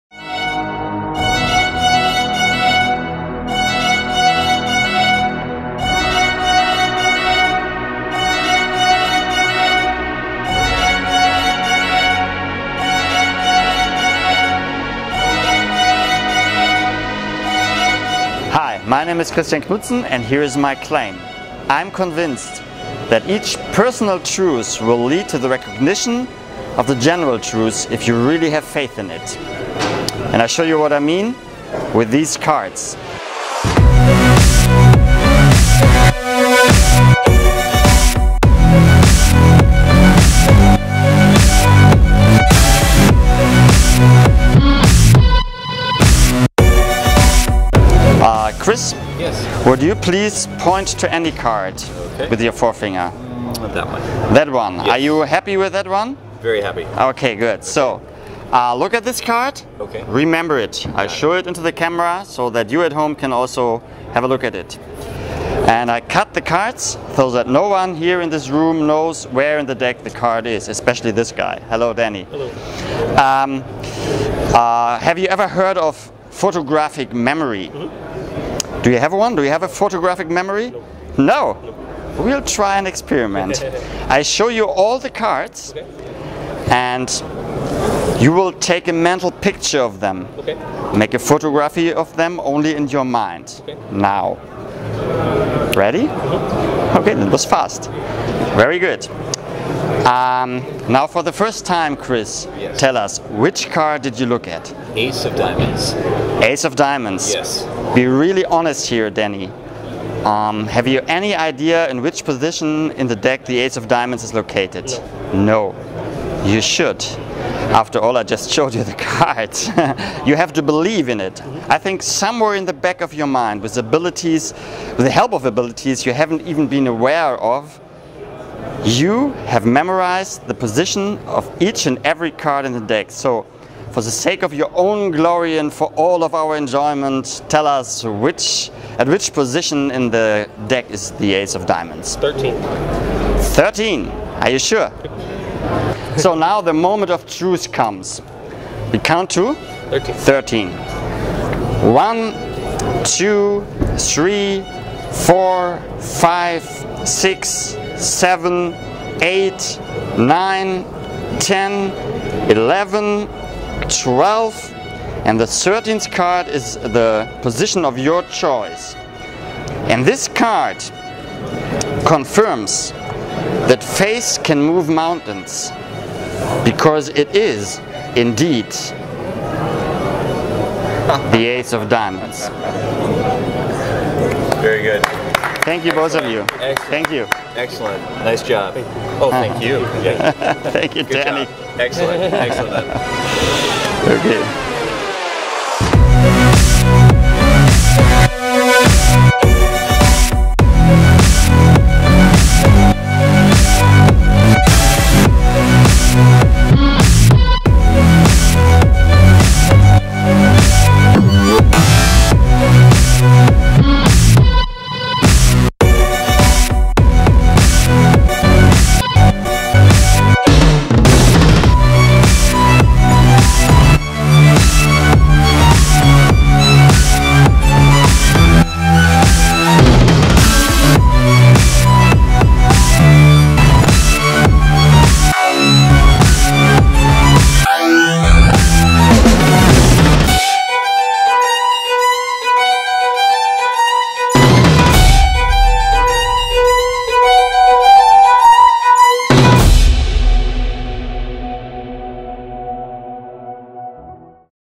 Photographic Memory - Christian Knudsen
We met with Christian at Nordisch Nobel in Lübeck and filmed the Demo for his awesome effect "Photographic Memory"!
Display the Entire Deck Openly; Fronts AND Backs
Use Any Number Named!
No Specially Printed Cards!
No Pre-Show Work!
No Deck Switches!
No Sleights Needed!
No Stooges!
No Calculations!
No Memorization!
Instant Reset!
Instant Miracle!